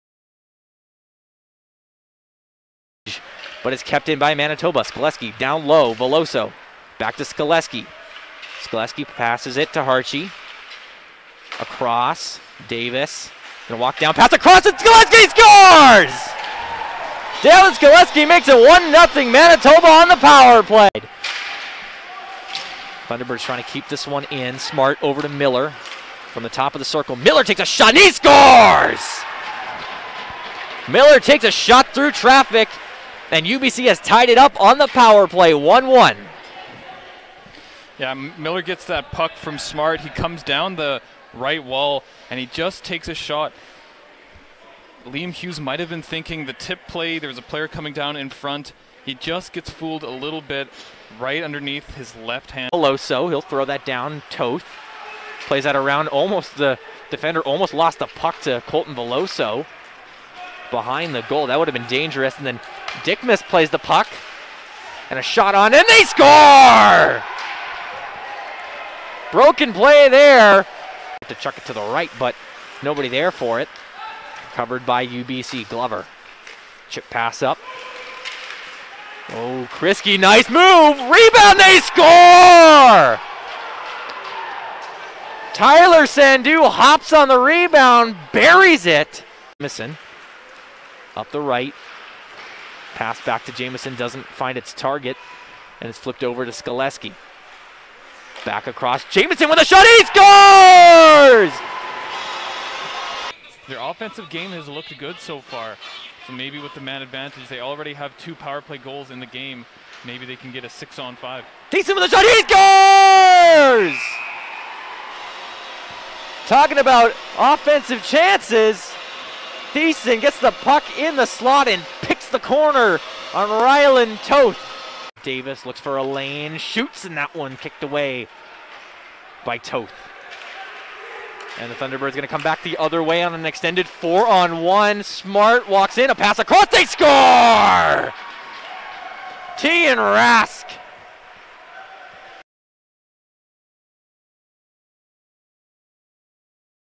Bisons Highlights | UBC 4, Manitoba 3 | October 22, 2021
The Manitoba Bisons lost to the UBC Thunderbirds 4-3 on Friday, October 22 at the Wayne Fleming Arena. Here are the highlights.